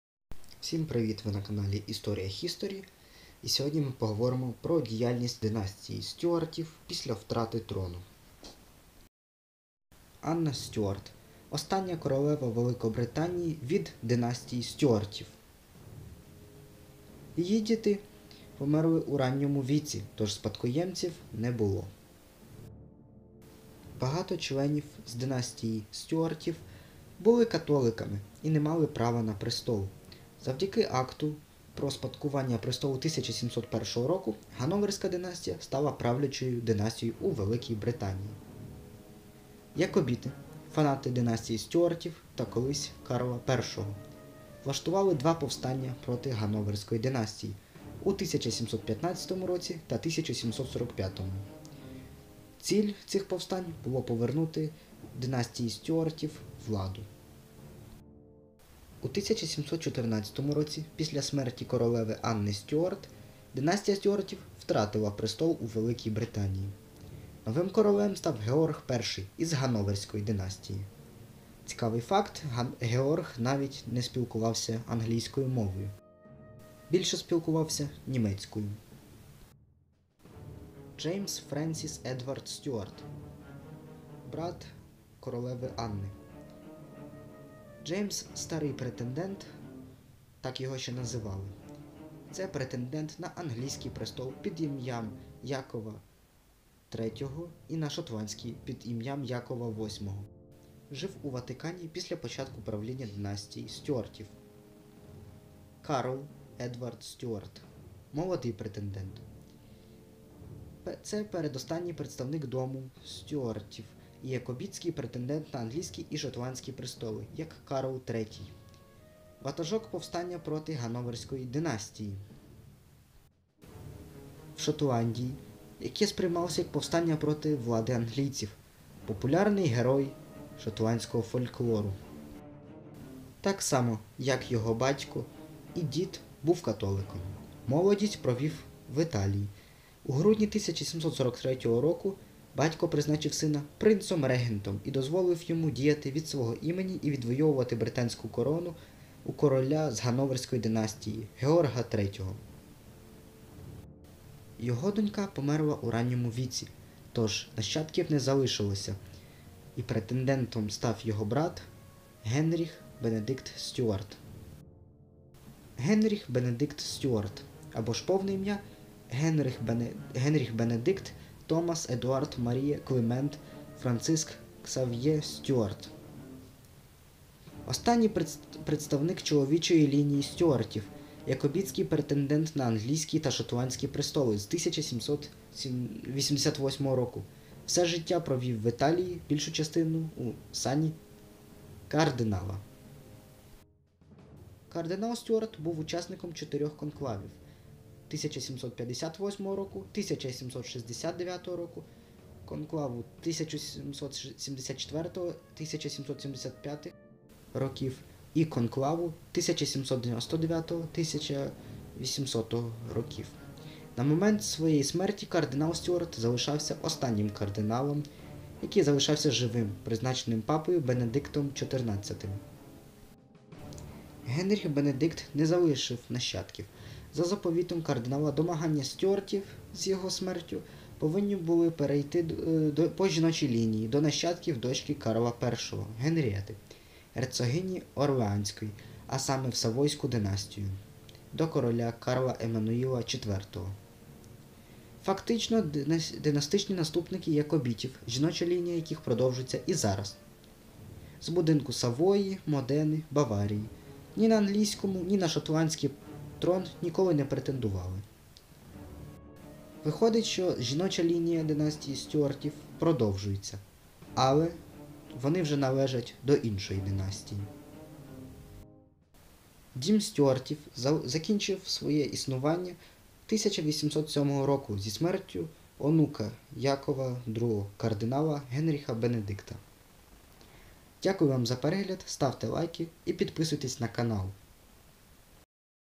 Стюарти після втрати влади у 1714 році | Історія | History
Історія династії Стюартів після втрати влади в 1714році.

Дякую вам за перегляд!